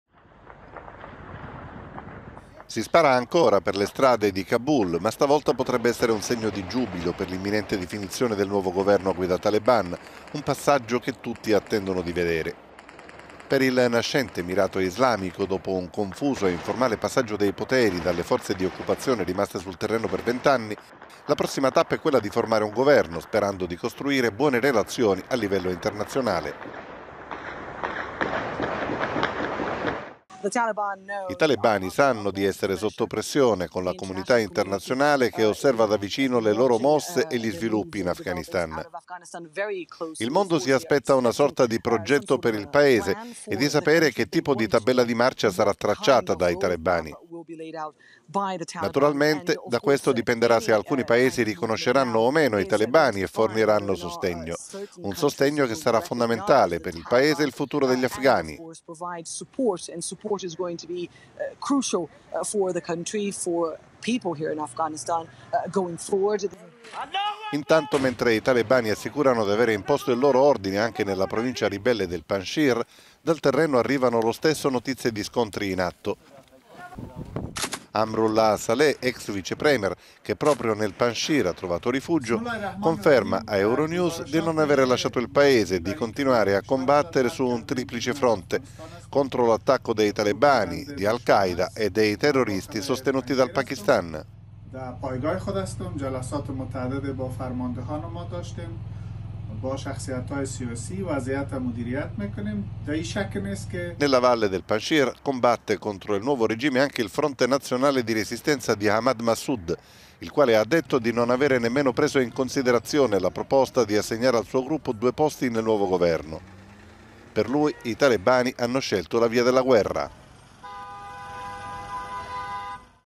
Kabul aspetta il nuovo governo talebano
Tra speranze e timori gli afghani seguono gli sviluppi del cambio di regime